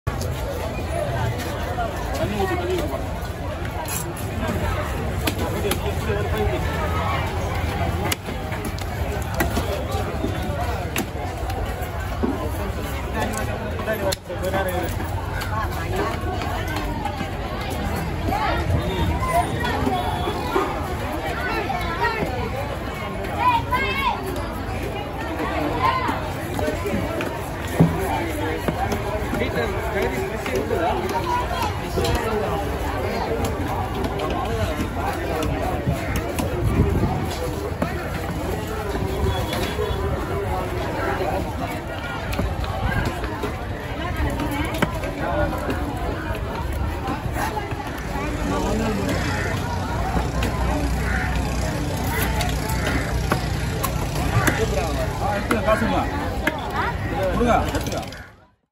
Massive Tuna Fish Cutting in Kasimedu Fish Market# Shorts
Here we have Showcased Tuna Fish Cutting in Kasimedu Fish Market, Chennai.